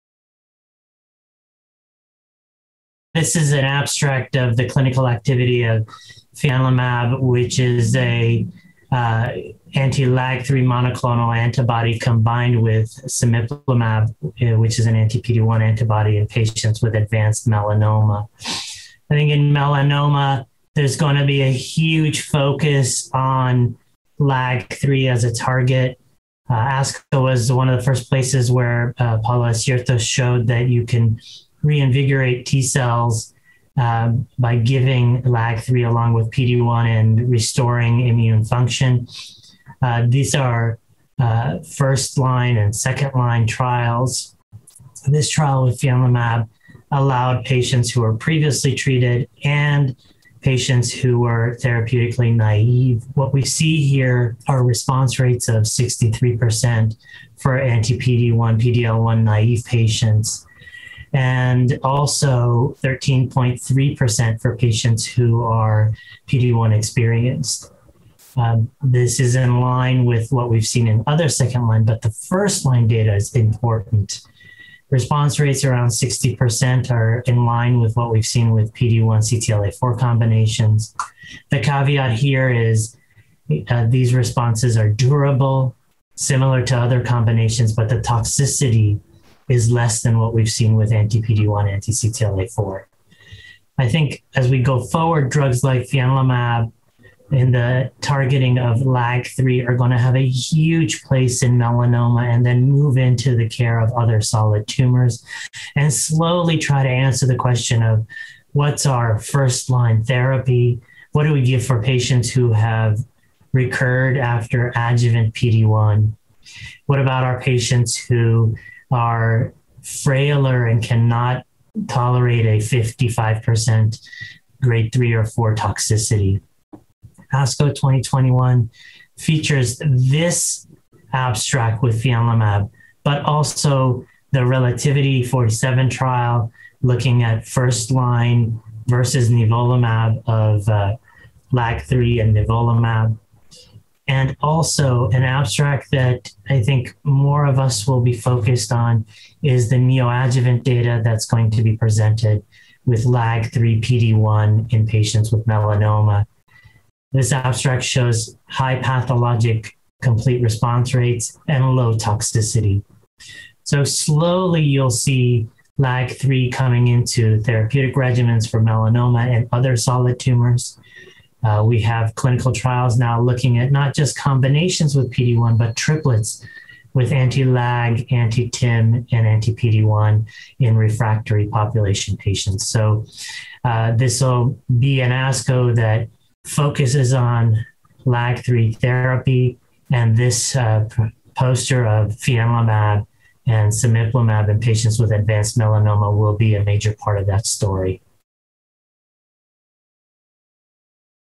Clinical activity of fianlimab, an anti-LAG-3 mAb combined with cemiplimab in advanced melanoma
Omid Hamid, MD, The Angeles Clinic and Research Institute, Los Angeles, CA, presents the safety and clinical activity data of a Phase I study evaluating fianlimab, an anti-LAG-3 monoclonal antibody (mAb) and cemiplimab in patients with advanced melanoma. The safety profile of fianlimab and cemiplimab was similar to that observed with cemiplimab monotherapy and the combination was associated with similar clinical activity to anti-PD-1/CTLA-4 approaches but with a more manageable safety profile. This interview took place at the American Society of Clinical Oncology (ASCO) 2021 Virtual Meeting.